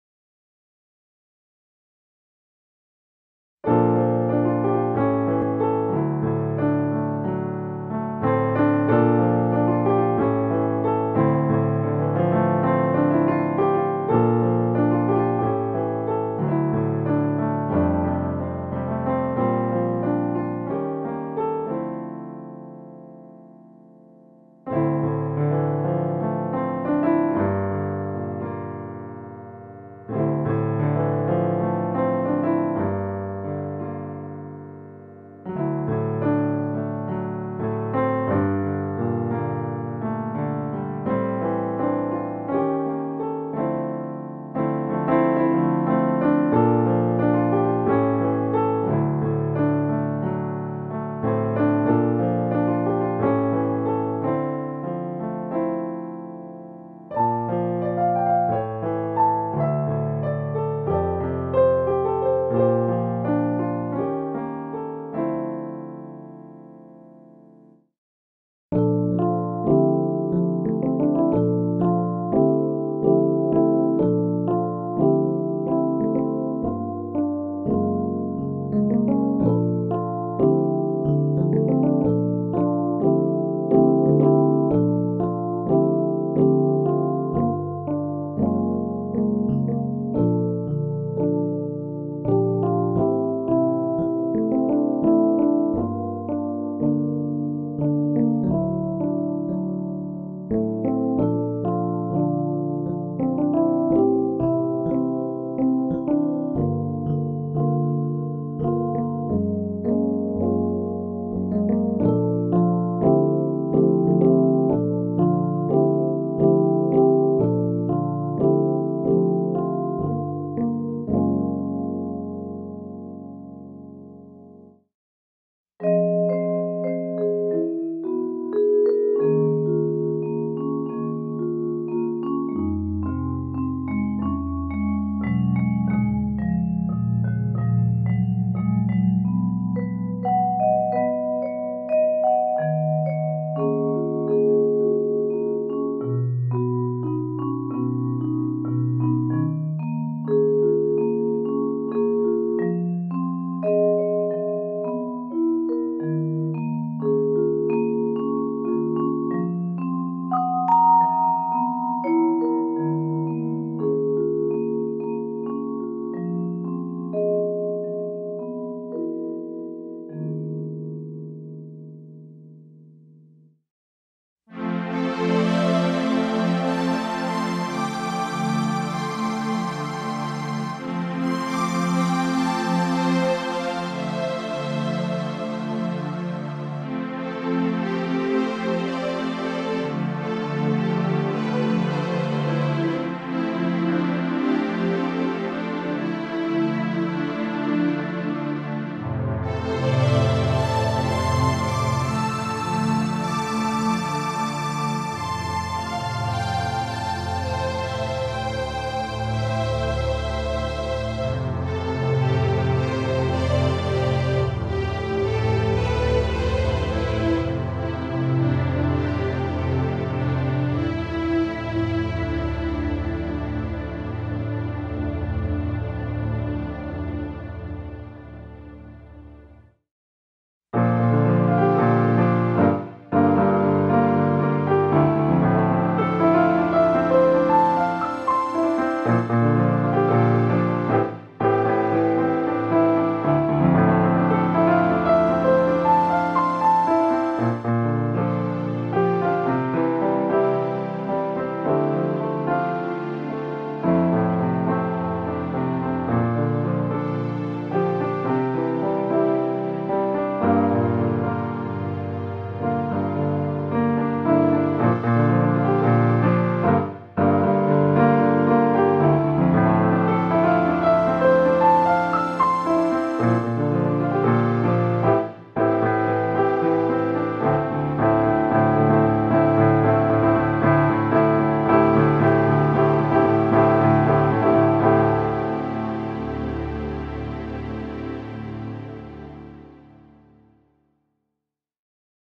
Best Selling Yamaha P45 Digital Piano - Keyboard Demo
Yamaha's P45 digital piano continues to be a leader in the industry and rightfully so.  With 88 weighted keys, 10 sounds, and computer connectivity, the P45 is loved by beginners and professionals alike.  Play A Note, Change Your Life!